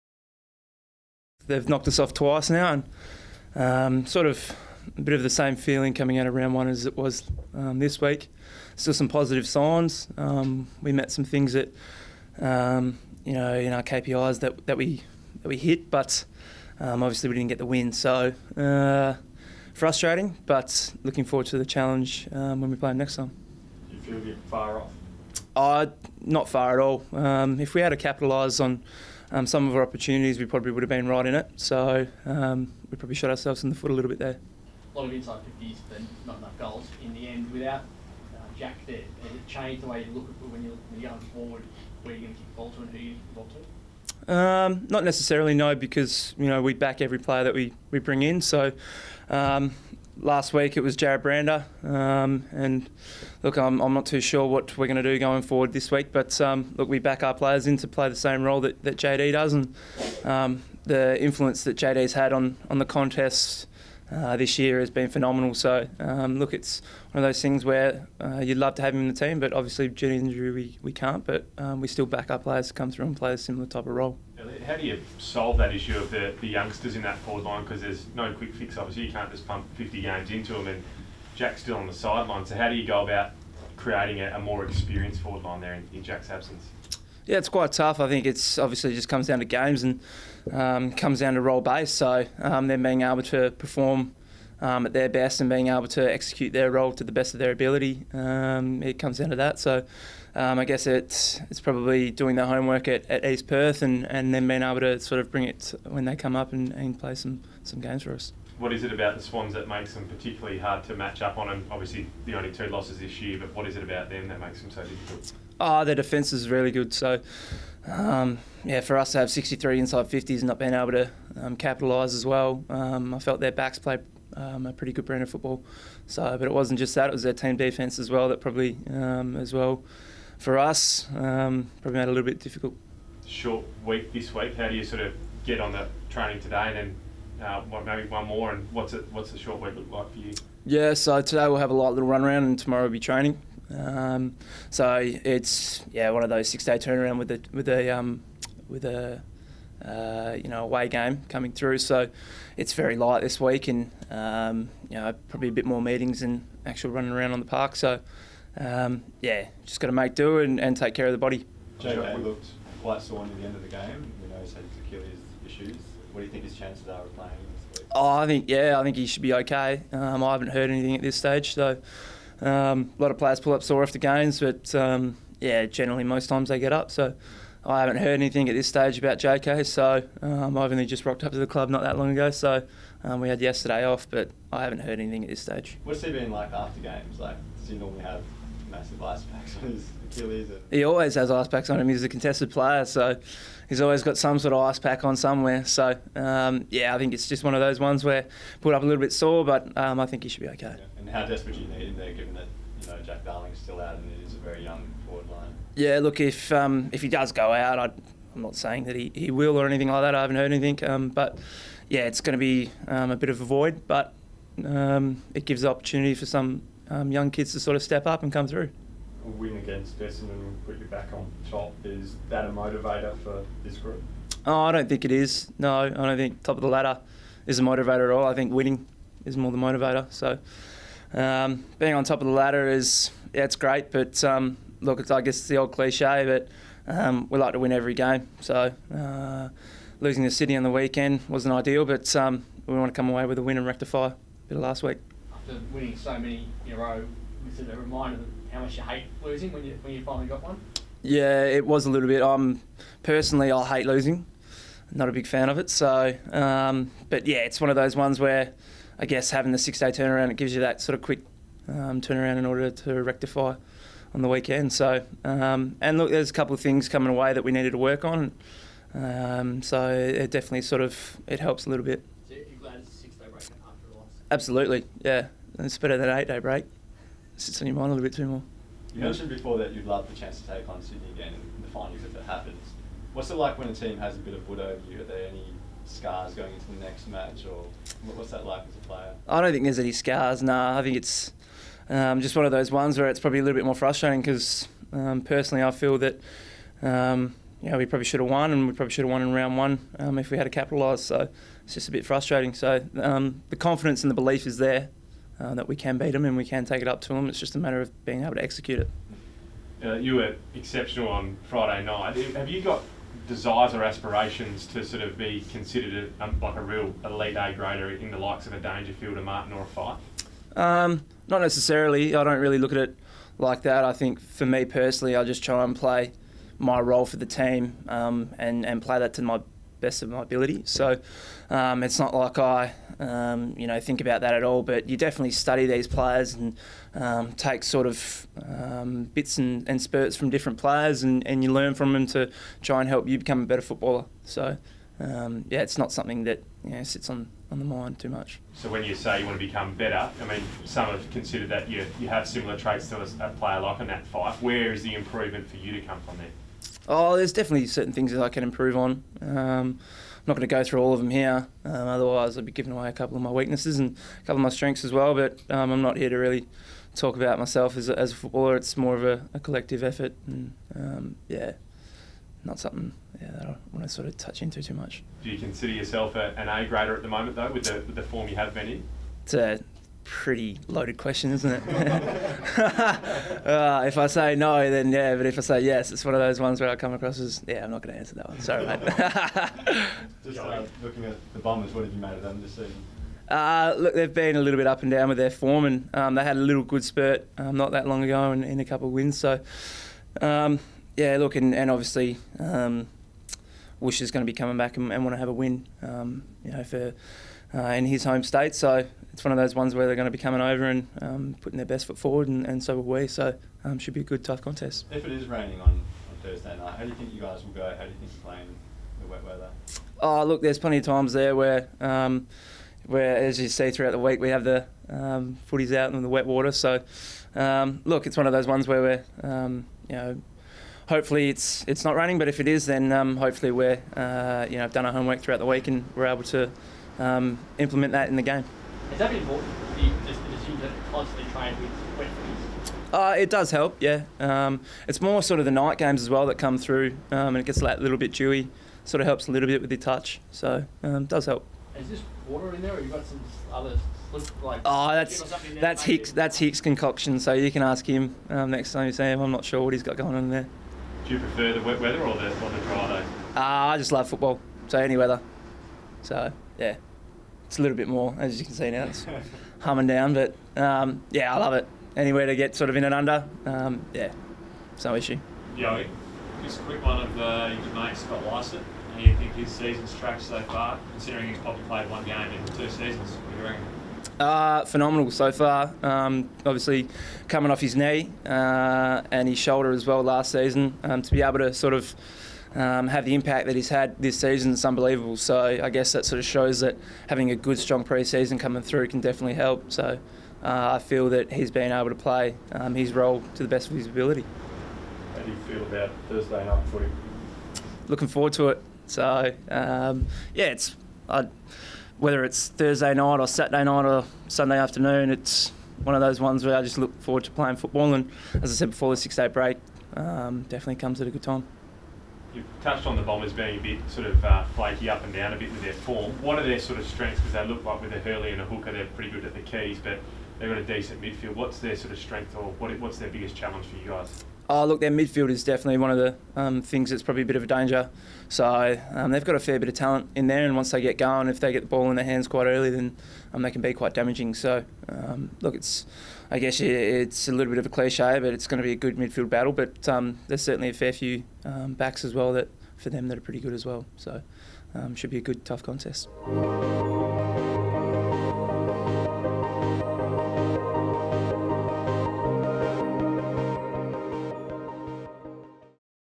Yeo press conference 18.06.18
The All-Australian Eagle speaks to the Perth media following his side's loss to the Swans